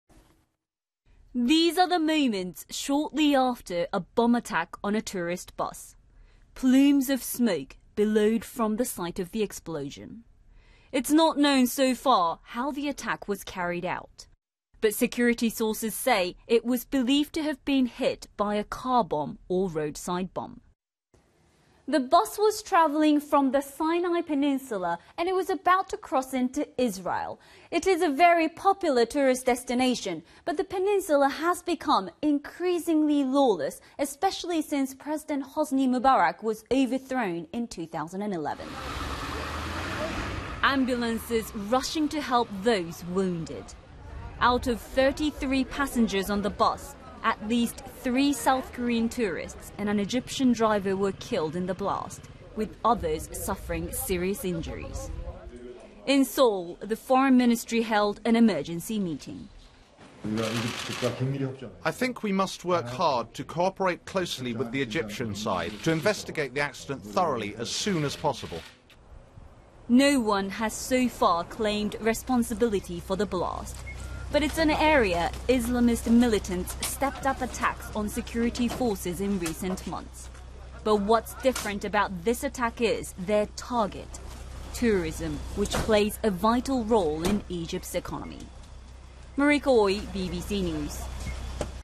Sinai attacks: Deadly bombing hits Egypt tour bus
A bomb attack on a tourist bus in the Sinai peninsula has killed at least three South Korean tourists and an Egyptian driver, officials have said.

A number of South Koreans were also hurt in the attack, which came as the bus was preparing to cross into Israel.

Security sources said the bus was hit by a car bomb or roadside bomb. No-one has admitted carrying out the attack.